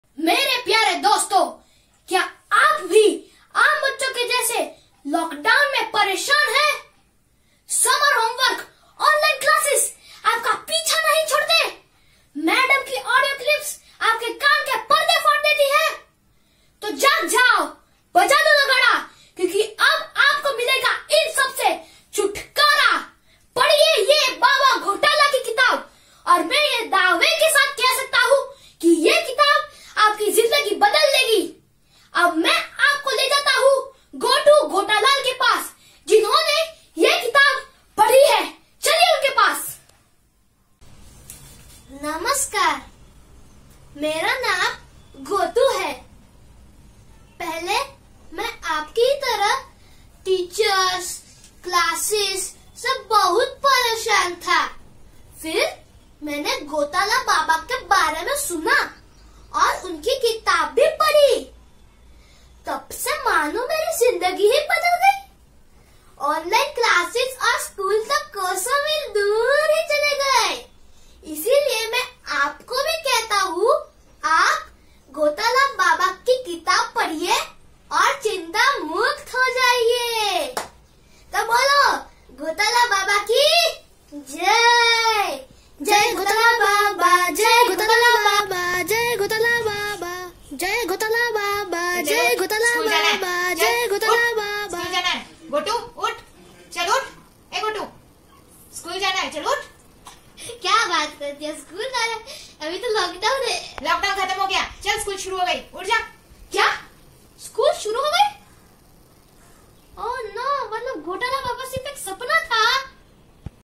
The Other Side - Kids activity during Lockdowns - By Yashraj Deshmukh. Funny Video
Kids been given homework and assignments during to Lockdowns.
Kids are versatile, they are curious as well comic. They have vivid skills and abilities.
The host of Know the things better showing his one of "The Other Side"